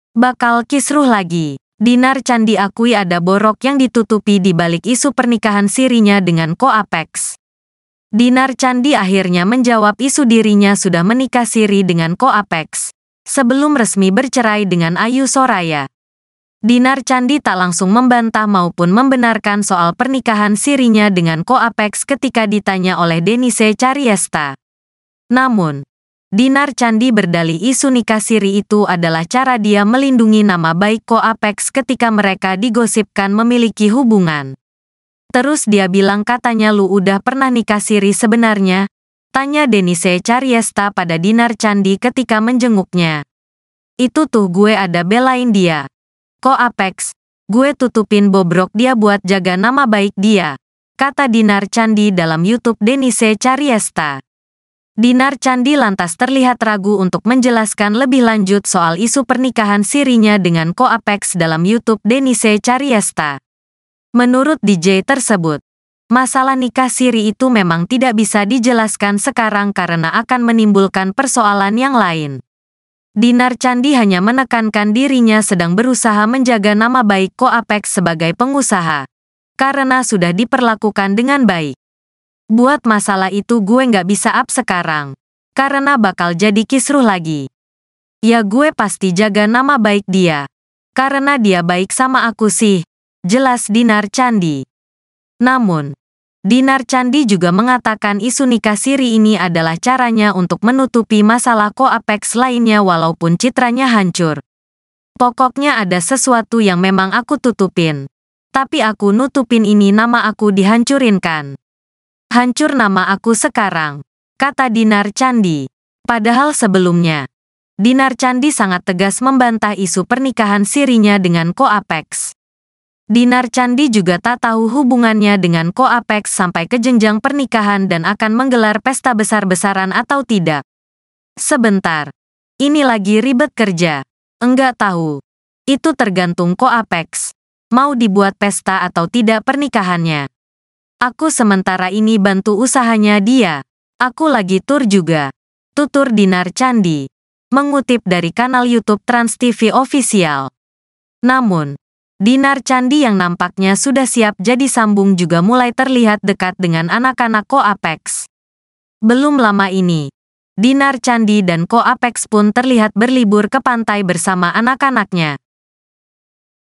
BAKAL KISRUH LAGI, DINAR CANDY AKUI ADA BOROK YANG DITUTUPI DI BALIK ISU PERNIKAHAN SIRINYA
“Bakal Kisruh lagi.” Dinar Candy Akui Ada Borok yang Ditutupi di Balik Isu Pernikahan Sirinya dengan Ko Apex.

Dinar Candy akhirnya menjawab isu dirinya sudah menikah siri dengan Ko Apex, sebelum resmi bercerai dengan Ayu Soraya. Dinar Candy tak langsung membantah maupun membenarkan soal pernikahan sirinya dengan Ko Apex ketika ditanya oleh Denise Chariesta. Namun, Dinar Candy berdalih isu nikah siri itu adalah cara dia melindungi nama baik Ko Apex ketika mereka digosipkan memiliki hubungan.

"Terus dia bilang katanya lu udah pernah nikah siri sebenarnya?" tanya Denise Chariesta pada Dinar Candy ketika menjenguknya.